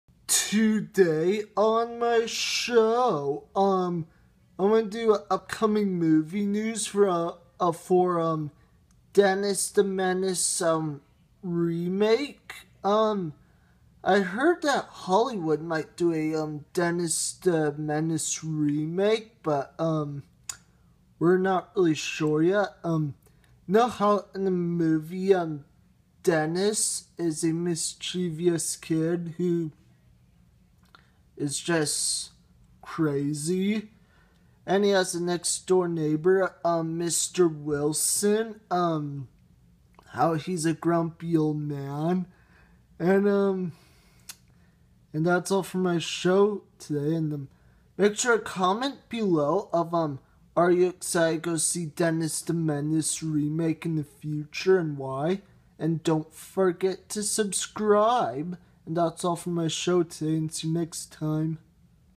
Upcoming movie news Dennis the Menace remake
'Dennis the Menace' remake lands writer (exclusive) based on the characters created by Hank Ketcham/based on the original writer by John Hughes of the original 1993 Walter Matthau movie Dennis the Menace (1993)